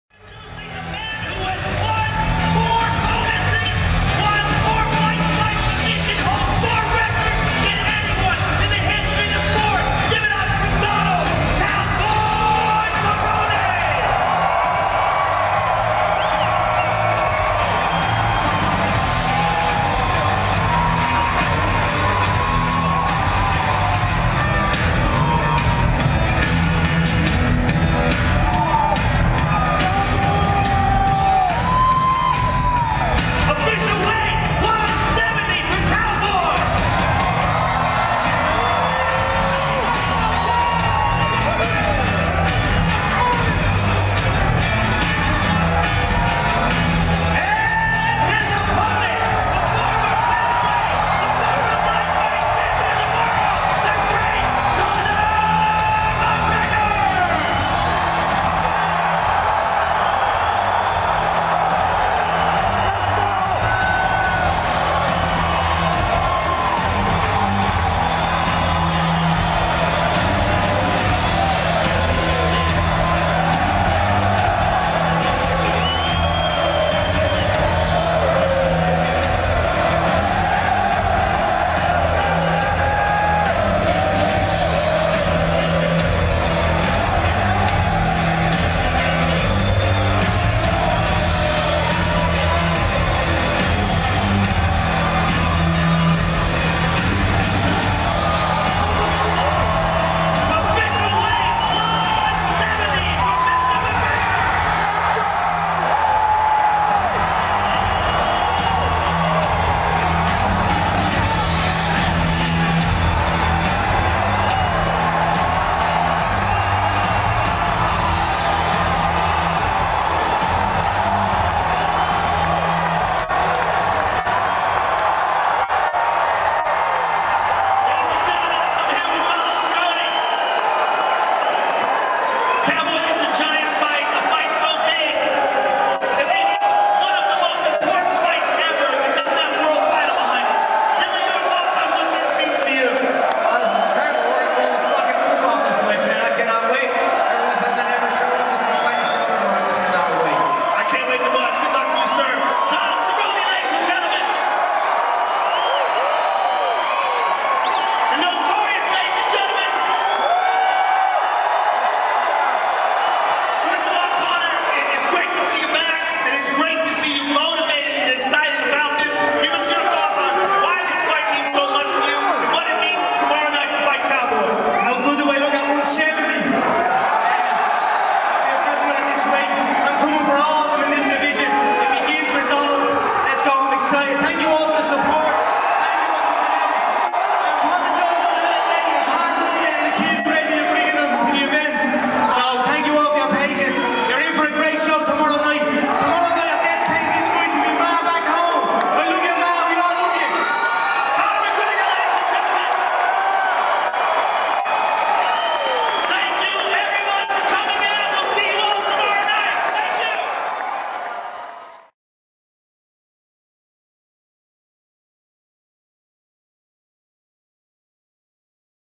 MCGREGOR GETS ALL UP IN CERRONE'S FACE; STARE EACH OTHER DOWN AT LIT WEIGH-IN & FACE OFF FOR UFC 246
FightHype.com was on hand in Las Vegas, Nevada where UFC stars Conor McGregor and Donald Cerrone held the weigh-in leading up to their highly-anticipated showdown in the main event of UFC 246. You don't want to miss what those in attendance had to say. Check it out!